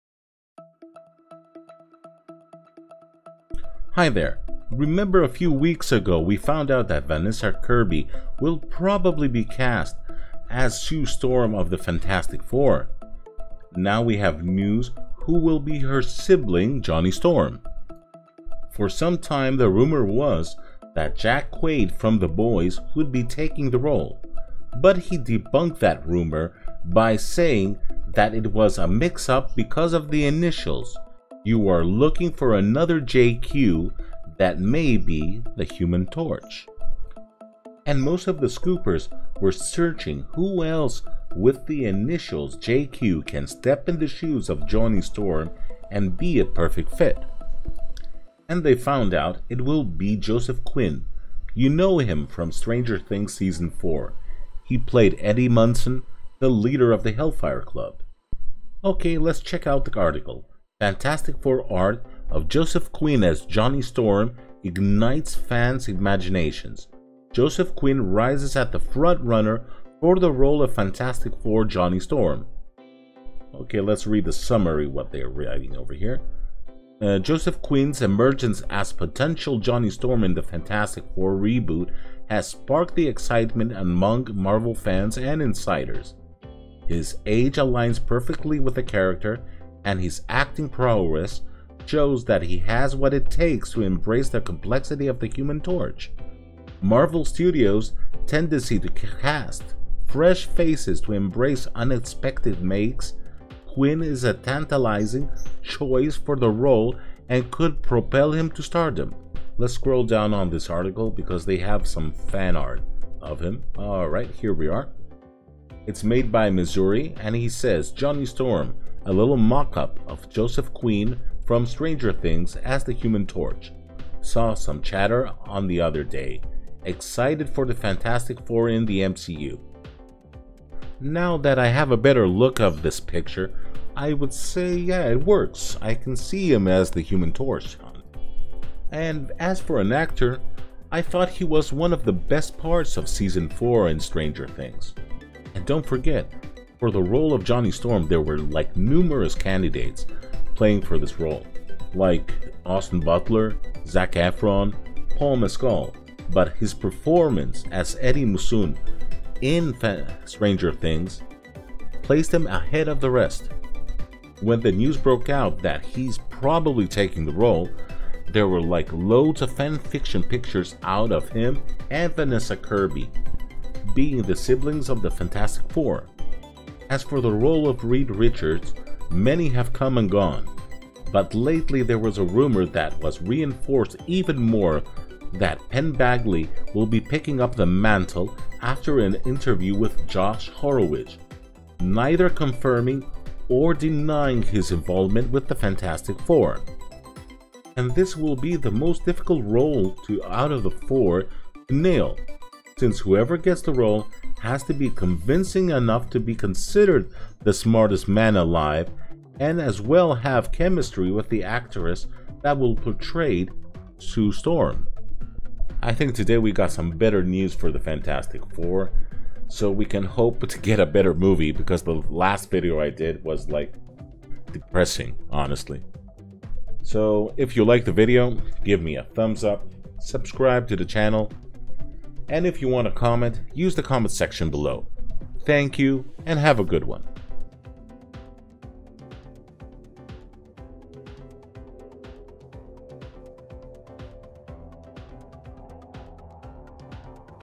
Major Casting Update Human Torch aka Johnny Storm & Mr. Fantastic Breaking news for Fantastic Four
New castings have come and they look relatively good lets see if they happen